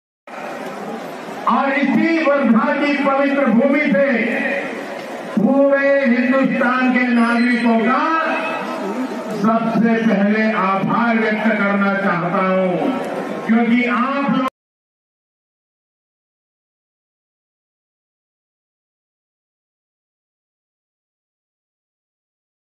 वर्धा येथील नरेंद्र मोदींच्या सभेत खुर्च्यां रिकाम्याचं !
पंतप्रधान नरेंद्र मोदी यांची महाराष्ट्रातील पहिली प्रचार सभा वर्धायेथे आयोजित करण्यात आली होती. यावेळेस  नरेंद्र मोदींनी विरोधकांवर हल्ला चढवल्याची चर्चा रंगली होती. मात्र आणखी चर्चा रंगली ती मोदींच्या सभेला असलेल्या रिकाम्या खुर्च्यांची. सभेसाठी पन्नास हजार खुर्च्यांची व्यवस्था करण्यात आली होती. मात्र, यातील अनेक खुर्च्या रिकाम्या असल्याचा व्हिडियो सोशल मीडियावर व्हायरल होत आहे.